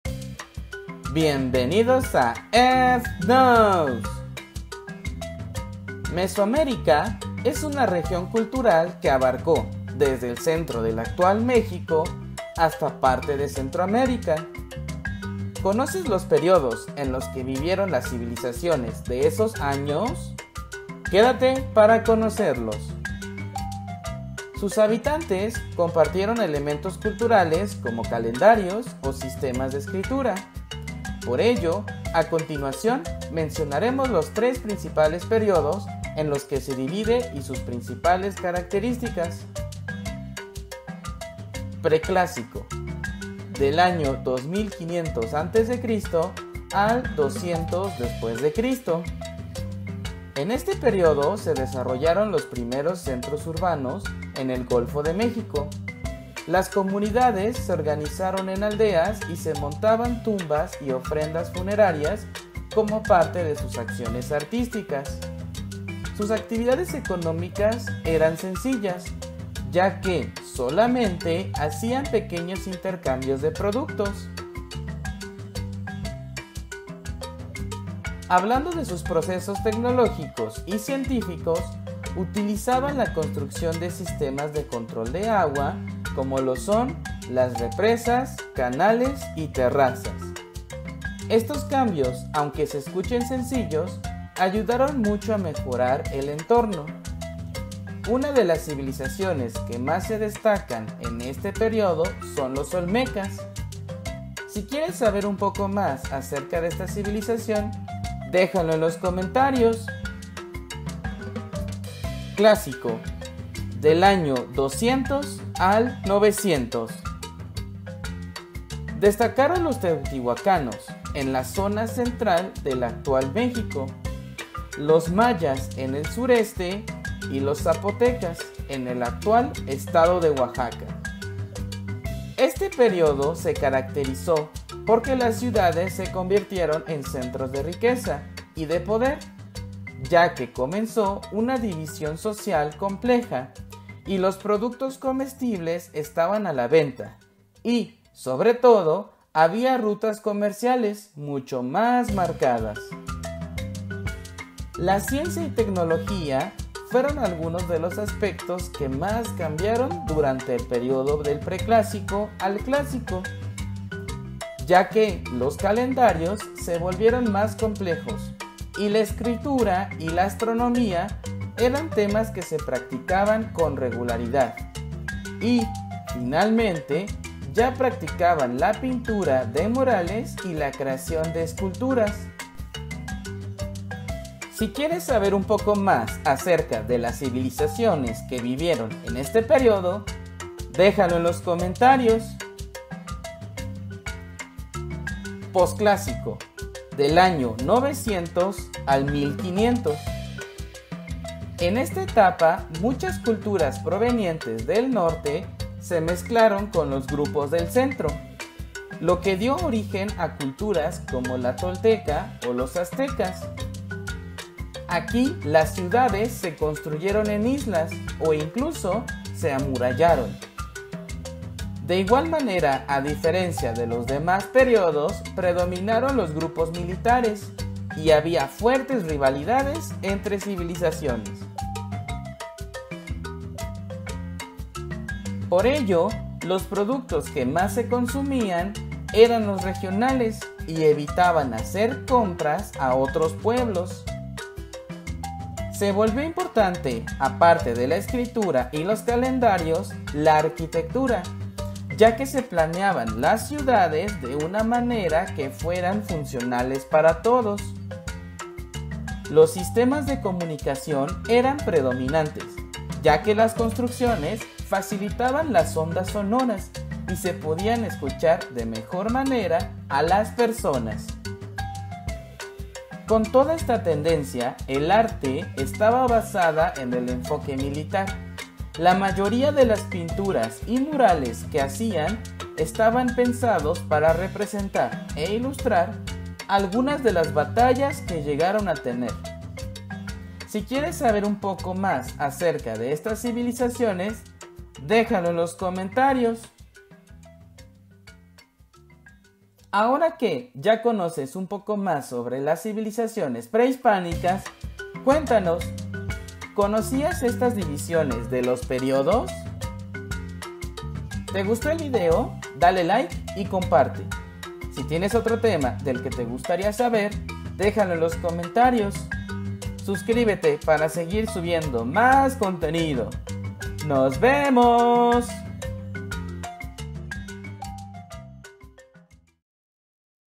Civilizaciones Prehispánicas (Preclásico, clásico y posclásico)
¡Estos son los periodos: preclásico, clásico y posclásico!
Te los explicamos resumidos.

Sí nuestros videos te ayudan puedes apoyarnos dándole like y compartiendo.

ILUSTRADORA: @hey_suss